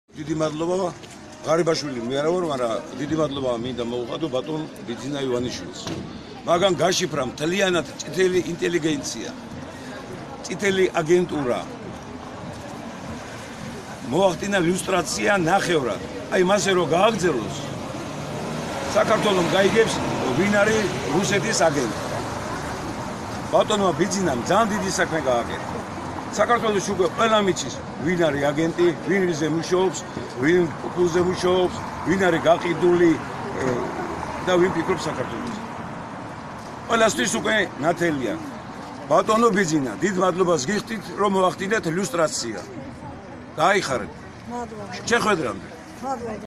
Gogi Tatukhashvili: ,,Mr. Bidzina! Before meeting!
TV. ,,RUSTAVI - 2 '' GEO. 2018.

Gogi Tatukhashvili: ,,Hello,

I am not Irakli Gharibashvili with his famous  phrase -thanks to Ivanishvili, but  anyway I would like to thank Mr. Bidzina Ivanishvili!

   He exposed the whole Russian-run red society, the Russian agents. Their lustration was held in half part. So to continue, Georgia fully understands itself  Russian agents.

     Mr. Bidzina has done a great job by making it clear to everyone today who works for Russia, who is an agent, a traitor, a true patriot and thinks for Georgia. Everything is clear to everyone!

    Again, thank you very much for this Mr. Bidzina!

    Before meeting!